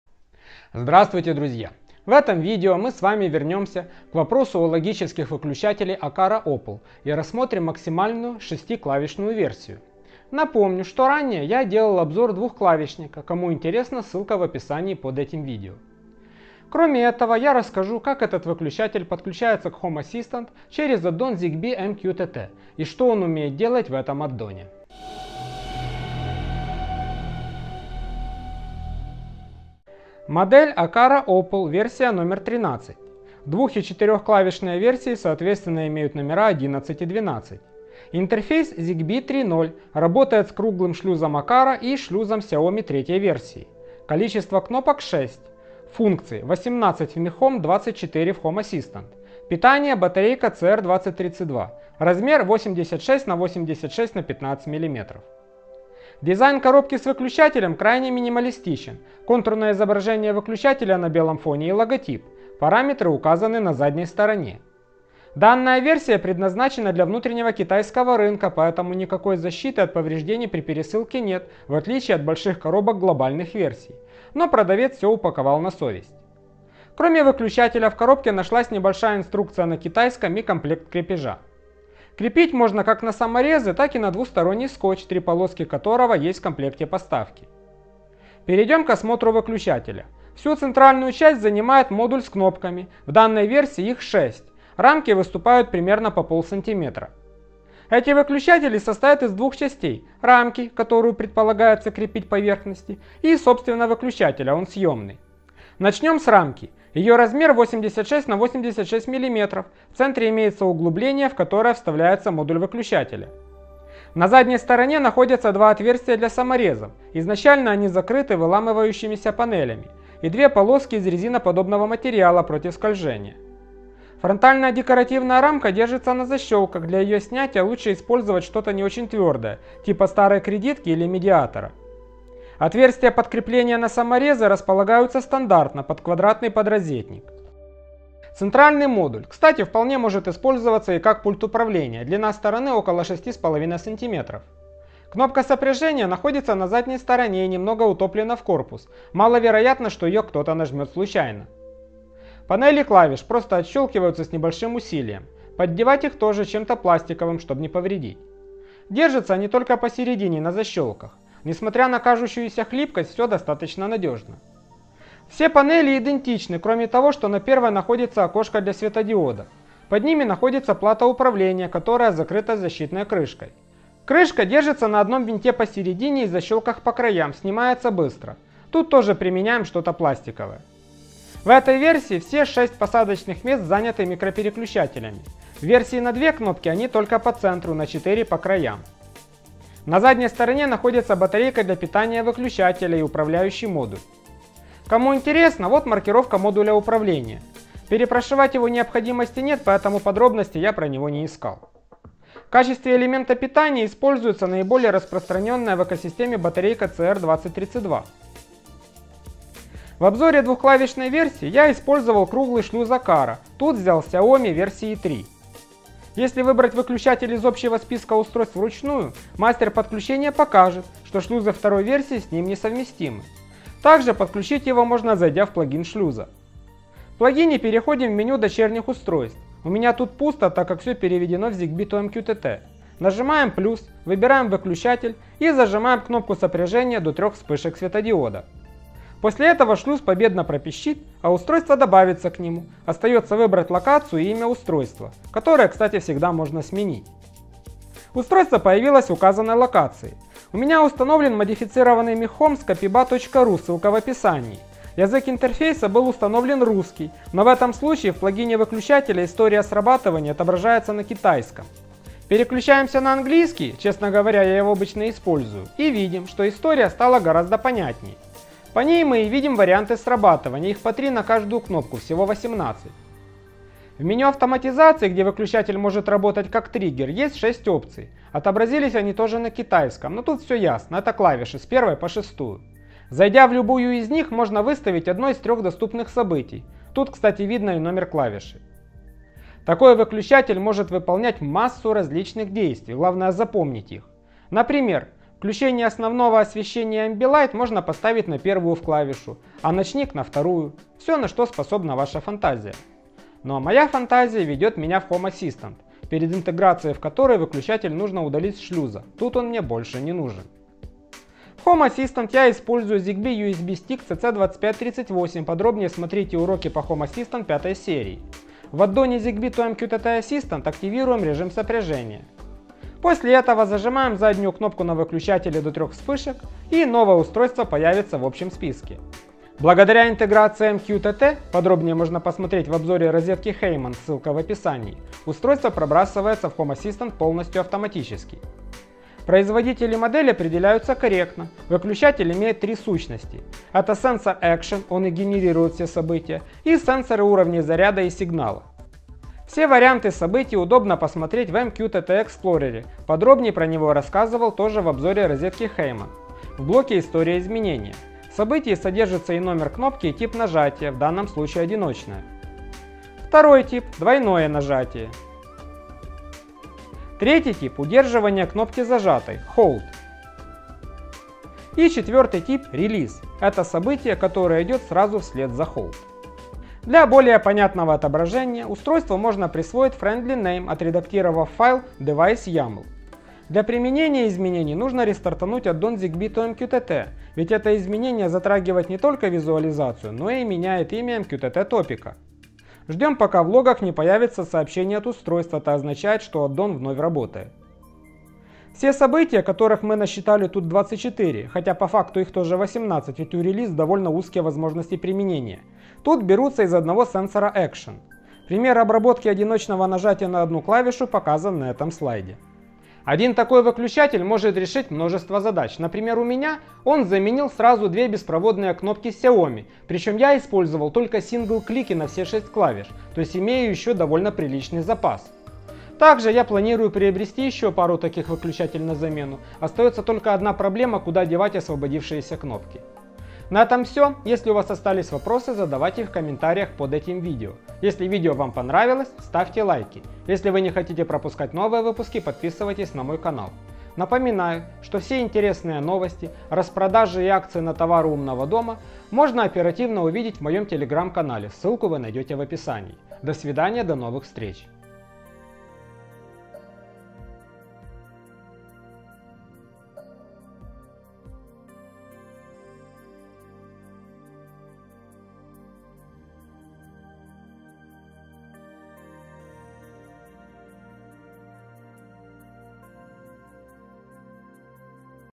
Aqara Opple - логический Zigbee выключатель на 6 клавиш и 18 действий
В этом обзоре мы рассмотрим максимальную - 6 клавишную версию Zigbee выключателя  Aqara Opple WXCJKG13LM. Я подключу его в Home Assistant и покажу что он умеет делать через аддон zigbee2mqtt.

Обновление от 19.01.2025 - Данная модель более не производится
 Актуальная бюджетная линейка - Aqara E1